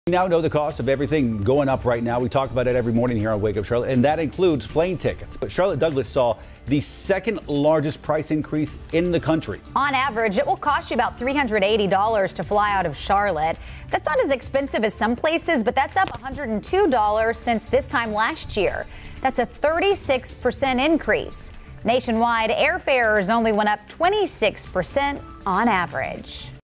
Airfare prices increase at CLT
Charlotte Douglas saw the second largest price increase in the country with it averaging at $380 to fly out of CLT.